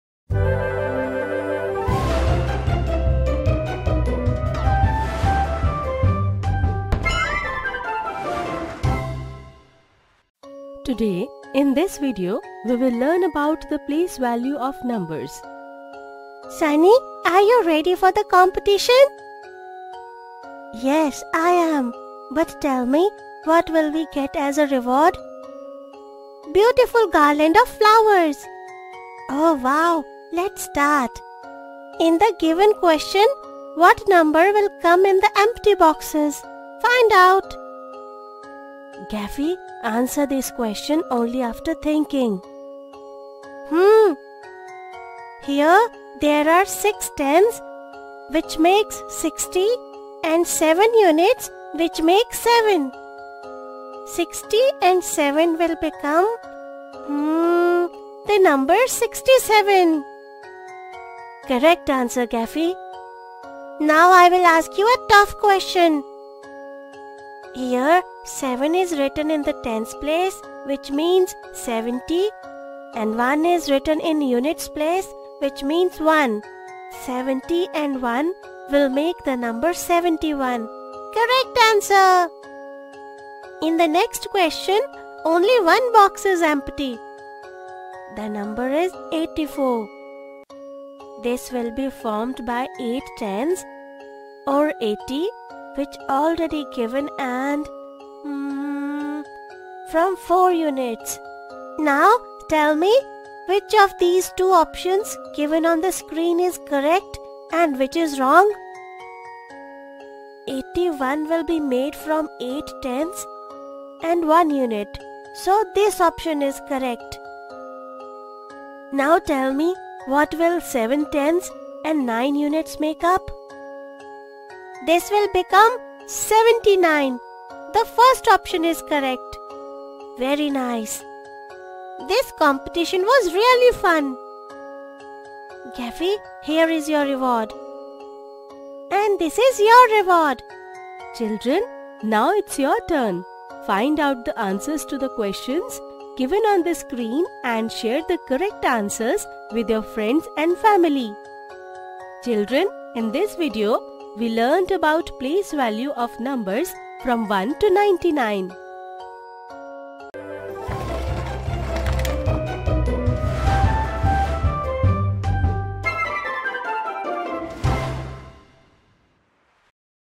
Place value of numbers from 1-99: Part 2| Number Sense| Ones| Tens| TicTacLearn| Fun with Gaffy
Dear students,

Happy Learning!

Gaffy
Fun with Numbers
Addition without carryover
Number games
Nursery
Foundational Numeracy
NIPUN Bharat
Numerals (1-99): Composes and decomposes numbers using 10s and 1s (pictorially and numerically)